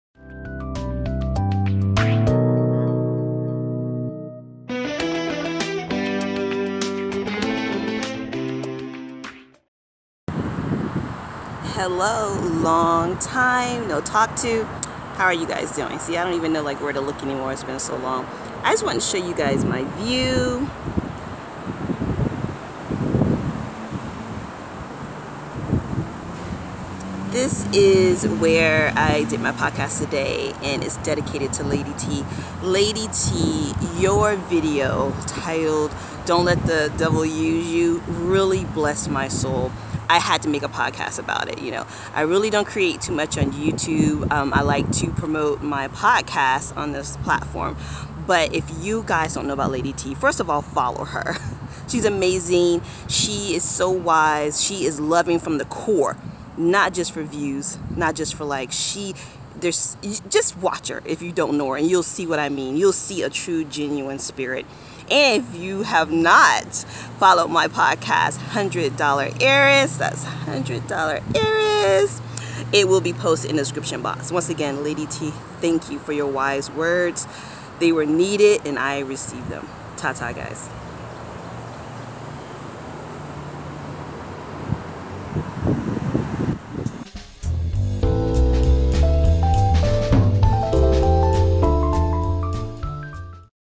Thank you Lady T!!
Lady T of The Flava Train put out a car video titled "Dont let the devil use you"! It hit hard with me. So much so I had to do a podcast about it. If you are asking God to promote you to the next level, I urge you to watch no LISTEN to it!